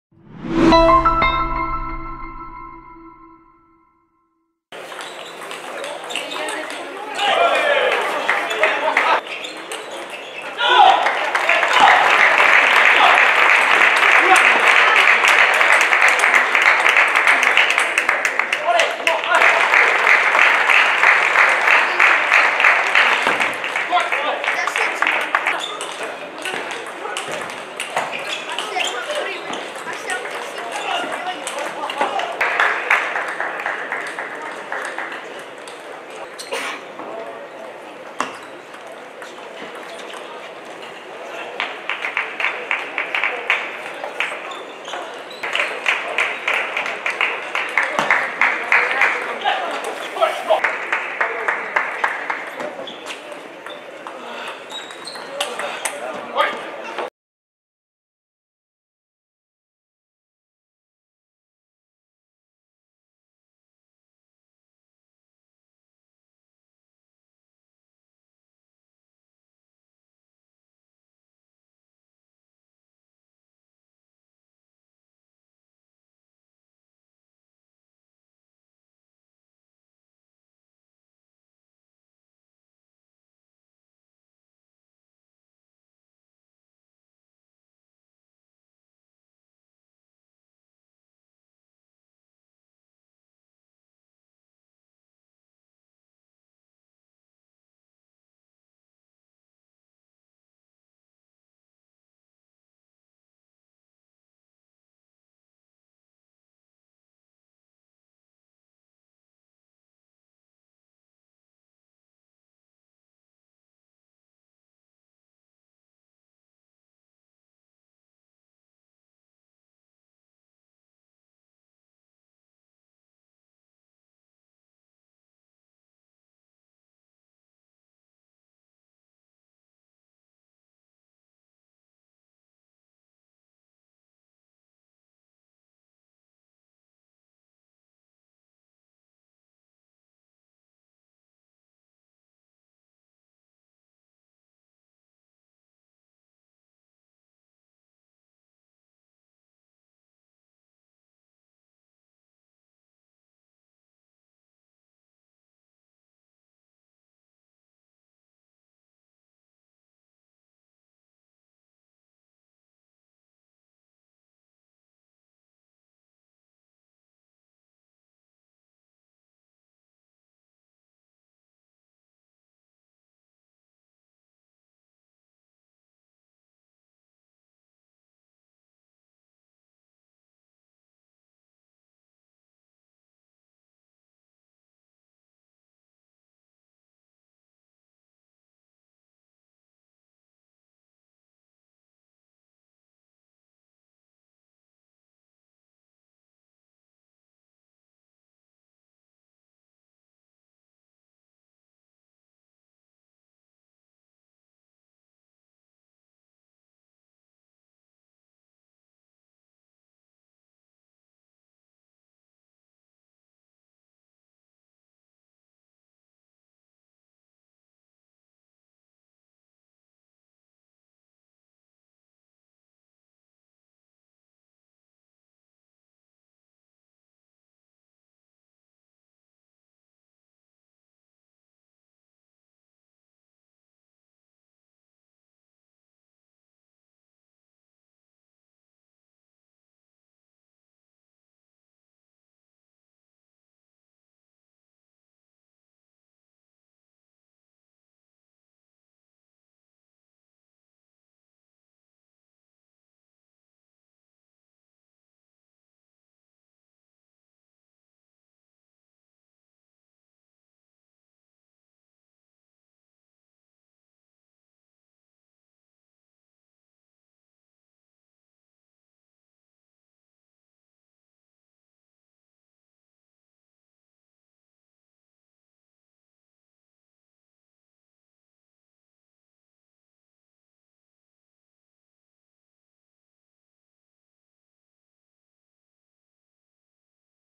Yu Ziyang vs Tomislav Pucar I 2018 ITTF Swedish Open Highlights (Pre)
Review all the highlights from the Yu Ziyang vs Tomislav Pucar from the 2018 Swedish Open